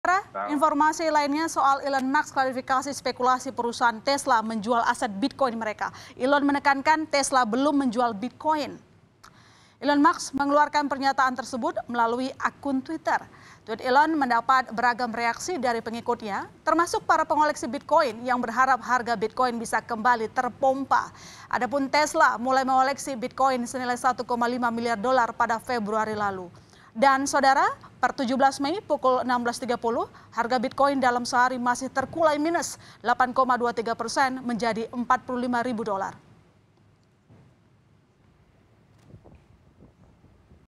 Elon Musk Klarifikasi Soal Tesla Jual Aset Bitcoin
KOMPAS.TV - Elon Musk klarifikasi spekulasi perusahaaan tesla menjual aset bitcoin mereka. Elon menekankan, Tesla belum menjual bitcoin.

Elon Musk mengeluarkan pernyataan tersebut melalui akun Twitter.

Tweet Elon mendapat beragam reaksi dari pengikutnya termasuk para pengoleksi bitcoin yang berharap harga bitcoin bisa kembali terpompa.

Adapun Tesla mulai mengolkesi bitcoin senilai 1,5 miliar dollar pada Februari lalu.

Dan per 17 Mei pukul 16.30, harga bitcoin dalam sehari masih terkulai minus 8,23 persen menjadi 45 ribu dollar.

Dilansir dari Kompas.com Potensi dijualnya bitcoin milik Tesla muncul selang beberapa hari ketika Musk berjanji bahwa perusahaan berencana menahan daripada menjual bitcoin yang sudah dimilikinya.

Dia bahkan menyatakan, akan menggunakan bitcoin untuk transaksi segera setelah transisi penambangan bitcoin berubah ke energi yang lebih berkelanjutan, bukan energi fosil.

Sebelumnya, Musk telah menjadi pendukung besar mata uang kripto, sehingga menjadi salah satu pendorong tingginya harga koin digital, termasuk bitcoin, beberapa kali dalam setahun terakhir.